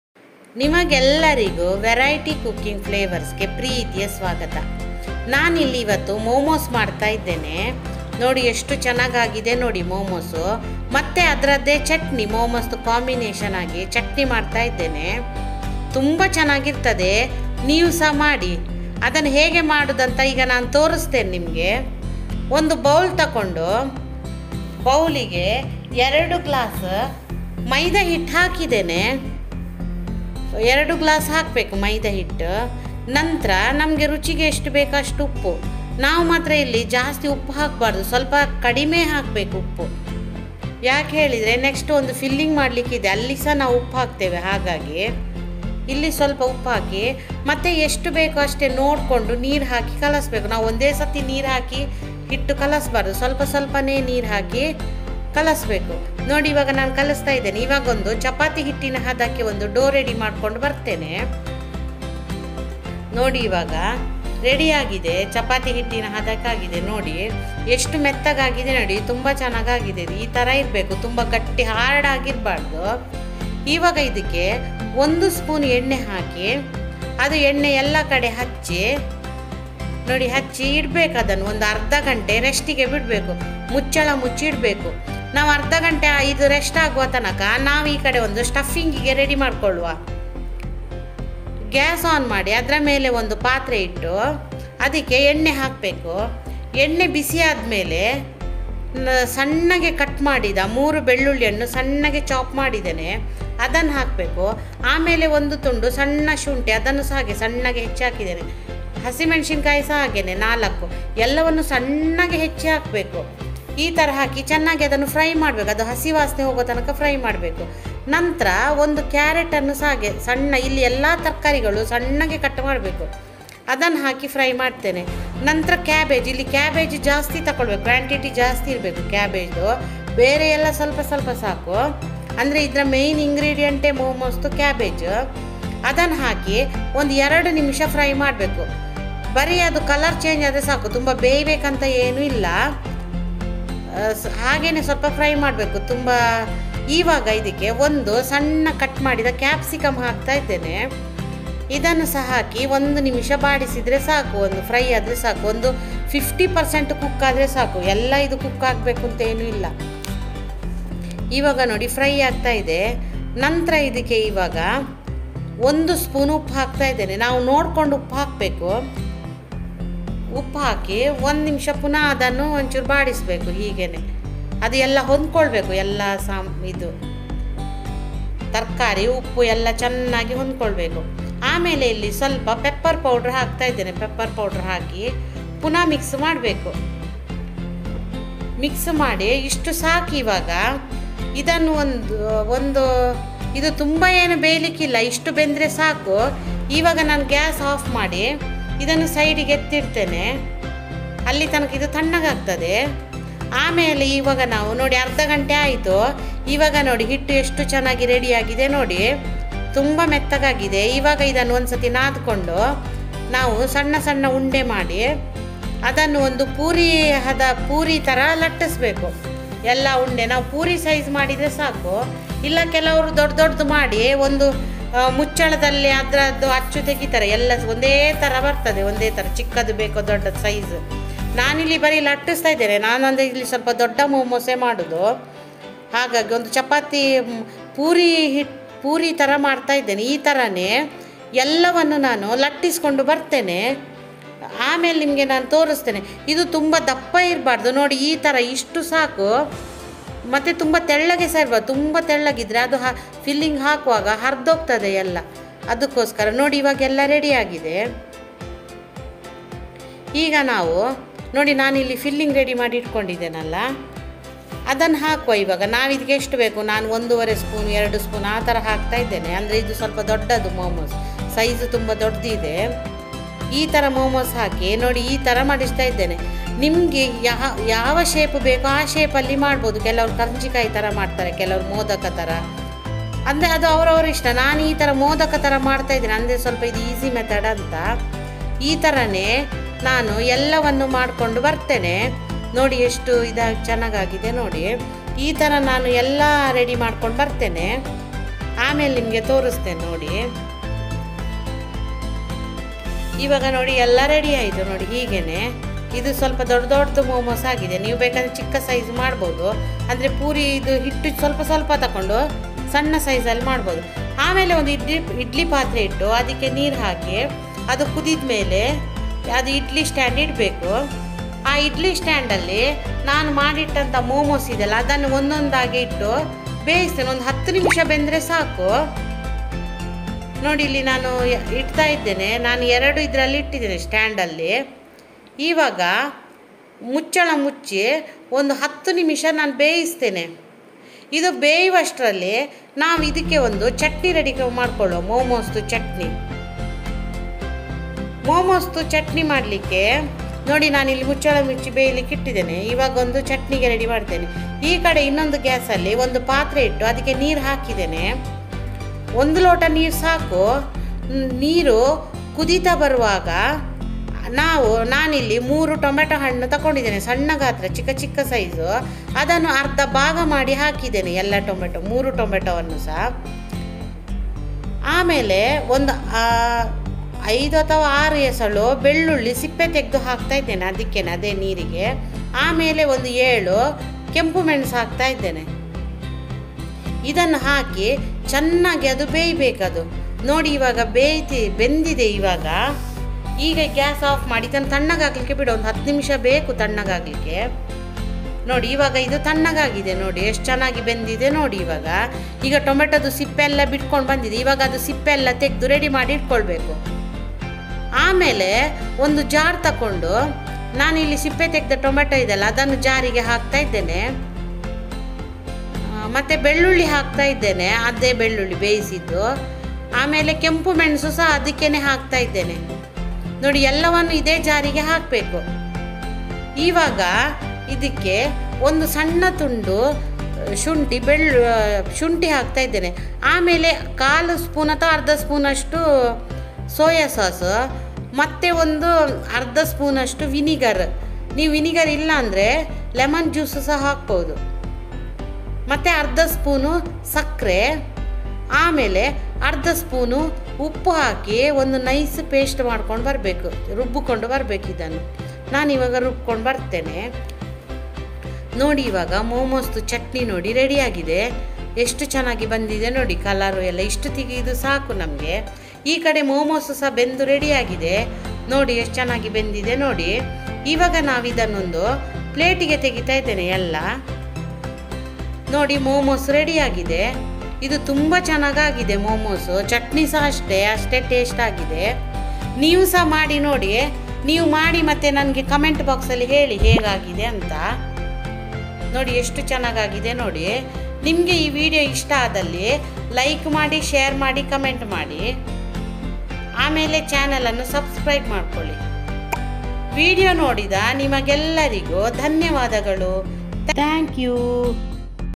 Veg Momo's with instant momo chutney.very tasty breakfast and snacks recipe.easy method
Variety Cooking Flavours vegmomos momochutney youtubeviralvedios vegpulau vegitablebath vegbiriyani famousvegbiriyami vegmomo chutney#
     Hai all
              Taday i prepare vegitable momos with instant momo chutney.very easy and simple method.very very tasty 😋.
   Variety Cooking Flavours#yoitube#vegmoms#momochutney#